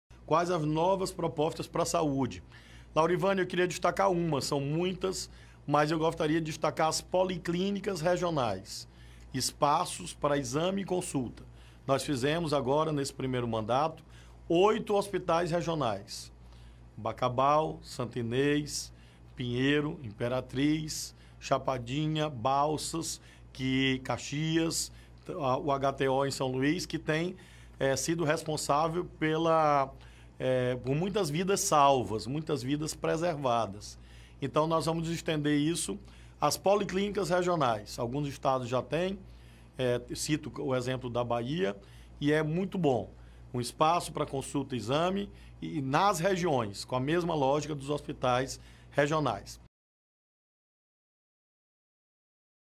Flávio Dino: “Vamos fazer as policlínicas regionais”
Governador fala sobre nova proposta para a Saúde, levando consultas e exames para todas as regiões.